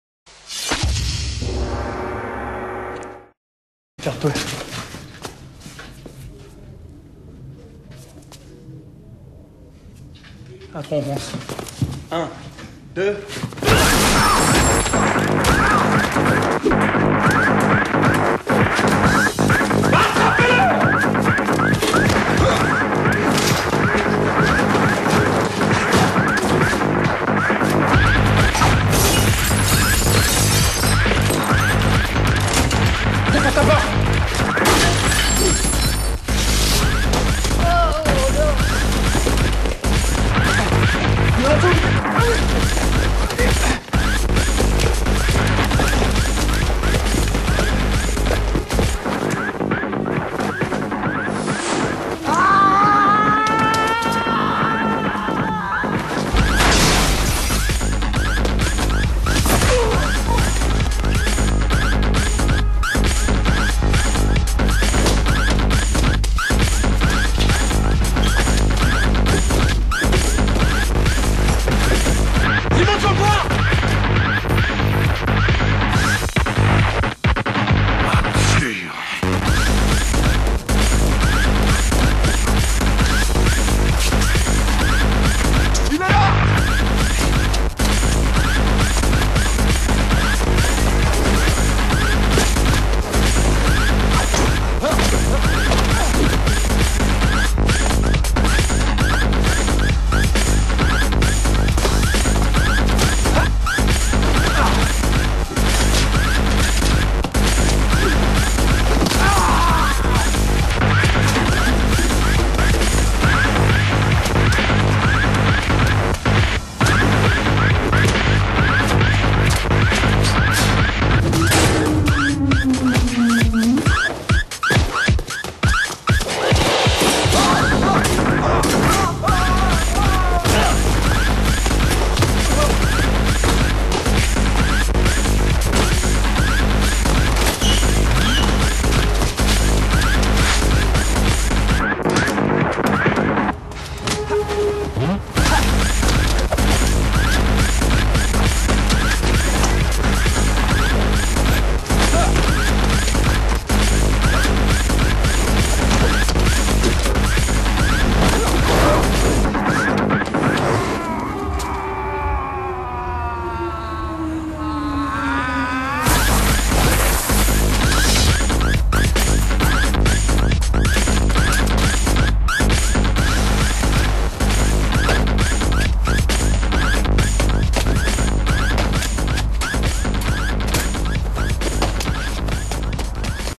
crazy french moves
this crazy french dude jumpin around.